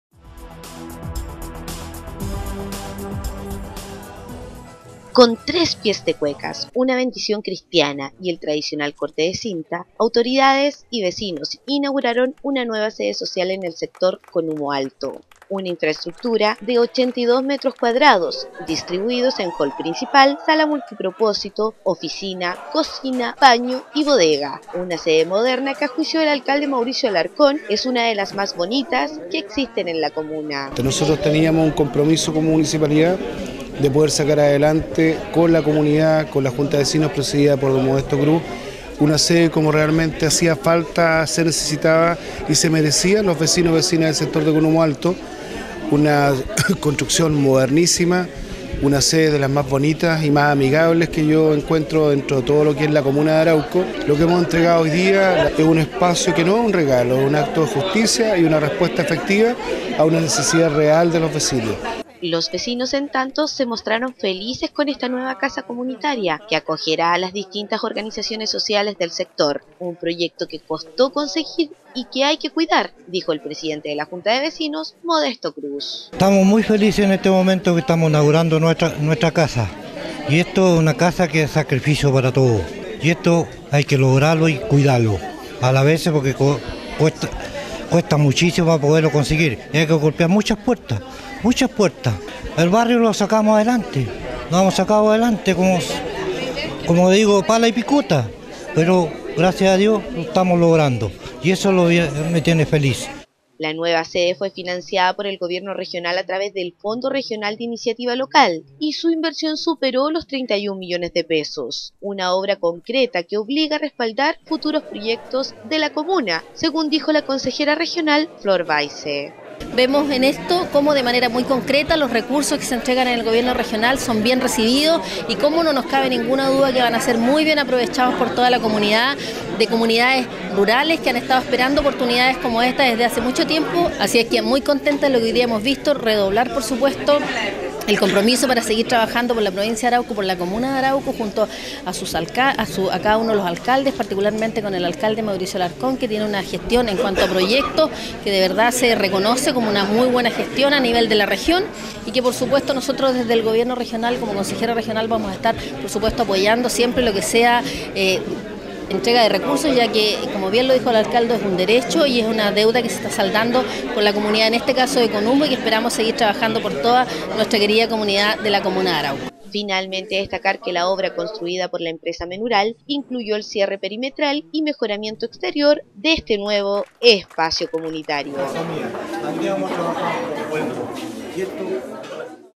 Se inauguró nueva sede social construida en Conumo Alto
Una infraestructura de 82 mt2 distribuidos en hall principal, sala multipropósito, oficina, cocina, baño y bodega. Una sede moderna, que a juicio del Alcalde Mauricio Alarcón, es una de las más bonitas que existen en la Comuna.